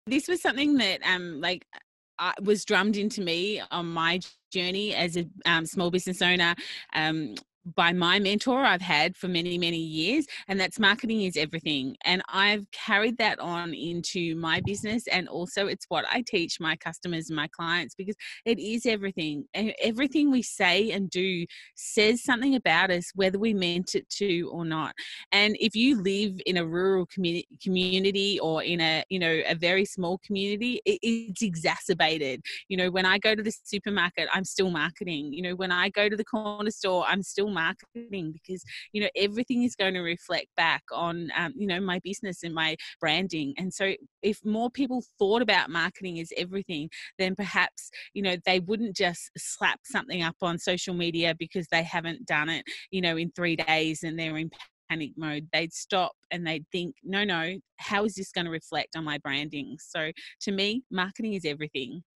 BizNation Ep17 - Marketing for Small Business Highlight #5
In this episode, we chatted with Jenn Donovan from Social Media & Marketing to unpack some basics about marketing for small business and how many of us (me included) can easily put this on the back burner and why we need to make it a priority.